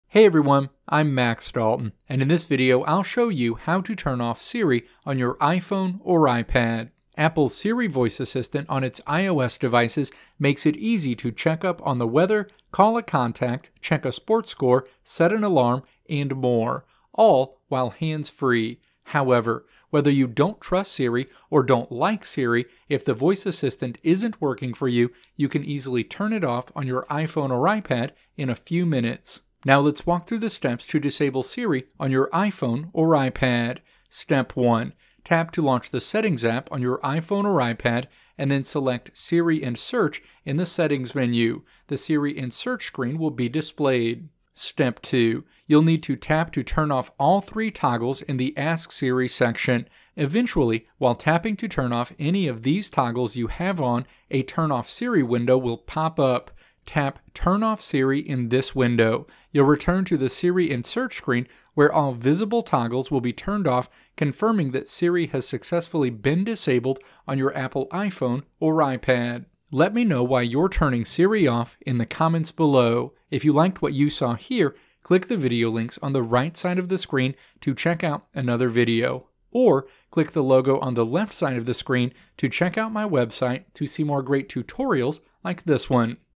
How to Turn Off Siri on iPhone or iPad
In this video I'll show you how to turn off Siri on your iPhone or iPad.

Timestamp:
Steps to Turn Off Siri on iPhone or iPad: 0:29

Video Transcript:
Apple's Siri voice assistant on its iOS devices makes it easy to check up on the weather, call a contact, check a sports score, set an alarm, and more -- all while hands free. However, whether you don't trust Siri or don't like Siri, if the voice assistant isn't working for you, you can easily turn it off on your iPhone or iPad in a few minutes.

Now let's walk through the steps to disable Siri on your iPhone or iPad.

Step 1. Tap to launch the "Settings" app on your iPhone or iPad, and then select "Siri and Search" in the Settings menu. The Siri and Search screen will be displayed.

Step 2. You'll need to tap to turn off all three toggles in the Ask Siri section. Eventually while tapping to turn off any of these toggles you have on, a Turn Off Siri window will pop up. Tap "Turn Off Siri" in this window. You'll return to the Siri and Search screen, where all visible toggles will be turned off, confirming that Siri has successfully been disabled on your Apple iPhone or iPad.